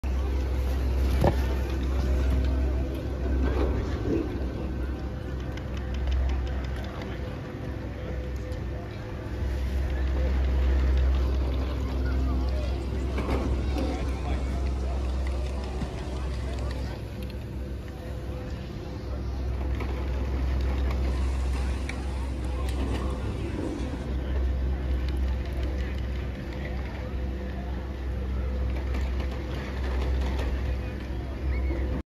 2011 Doppelmayr UNI-G-S Drive Terminal
This is the bottom drive terminal of the Monadnock Express Quad @Wachusett

2011 Doppelmayr Uni-G-S type terminal